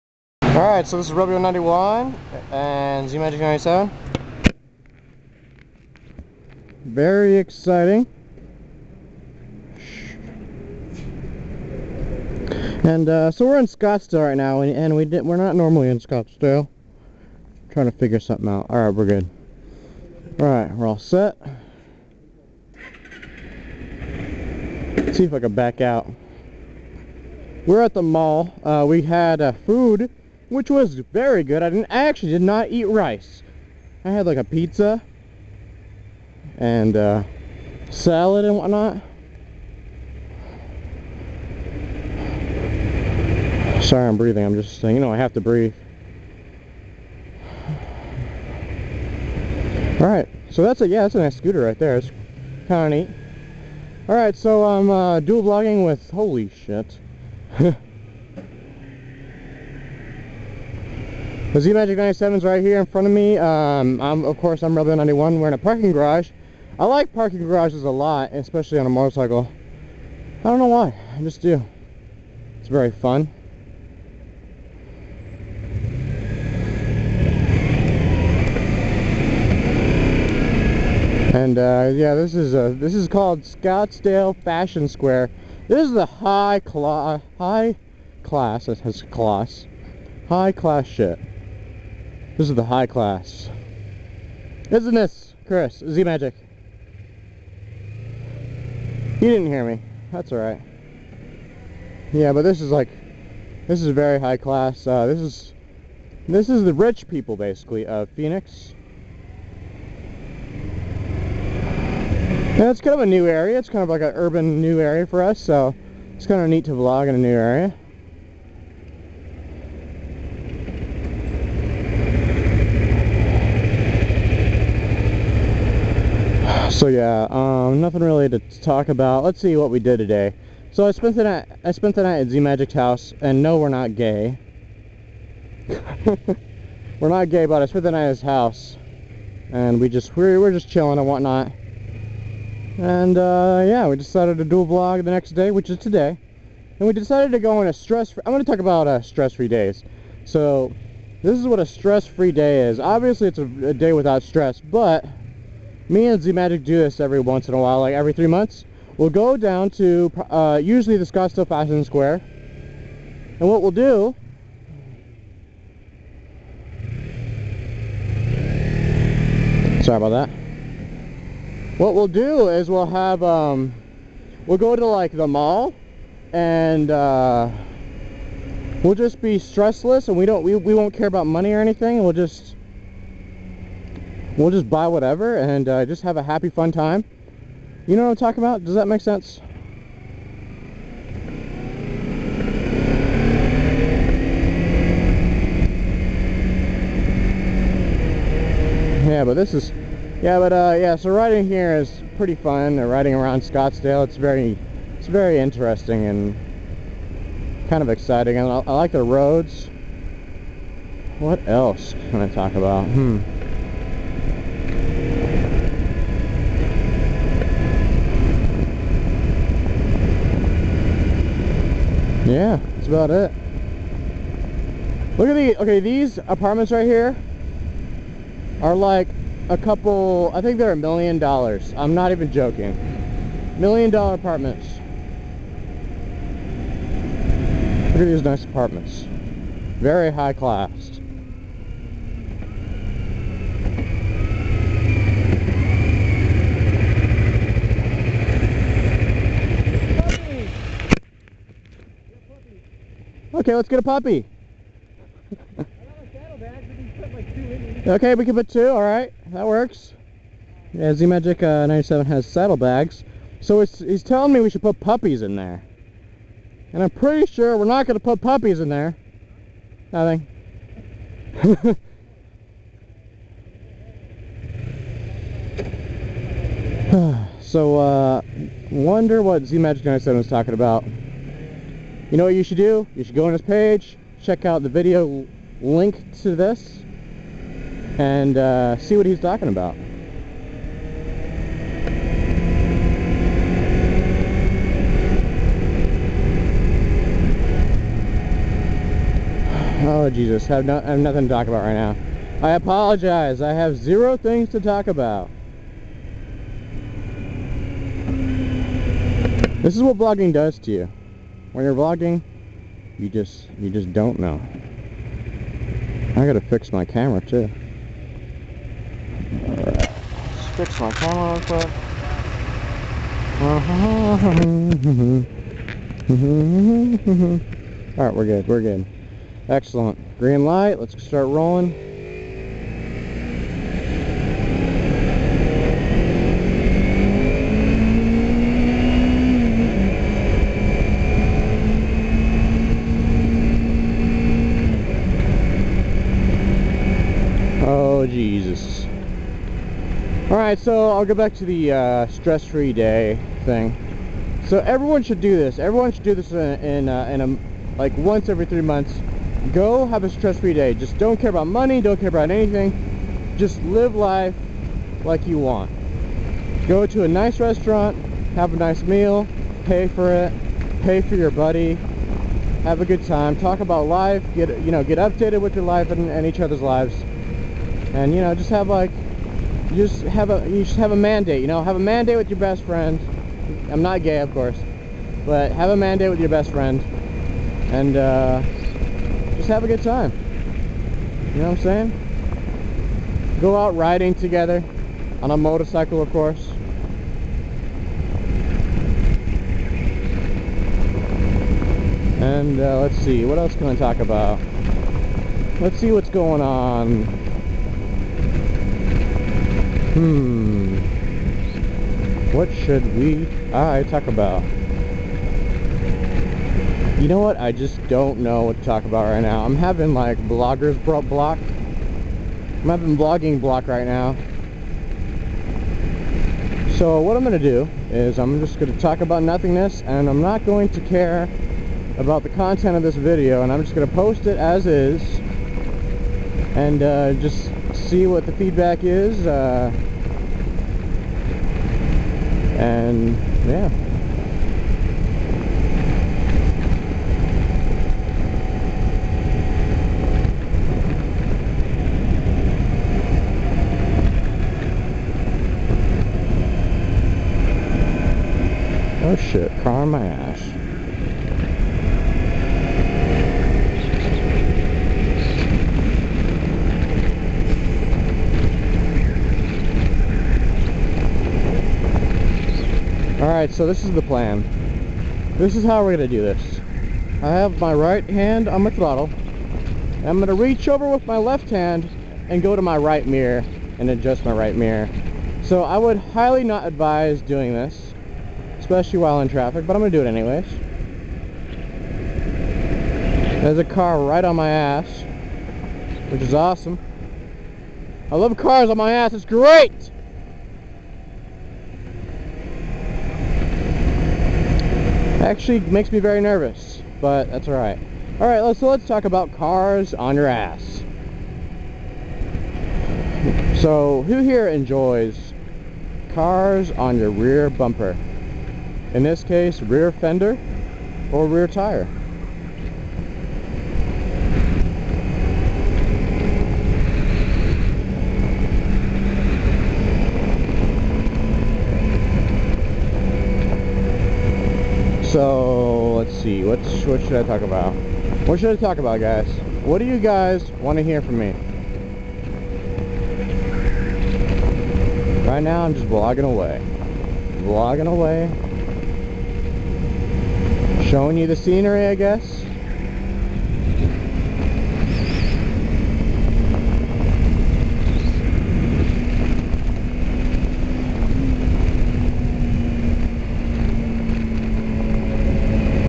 Cruizin with Zmagic97 - DV Part 1
Sorry about the cursing.

Also, I was gonna post this video as a whole 30 min mini movie but I tried Three times to upload it and they all failed. :P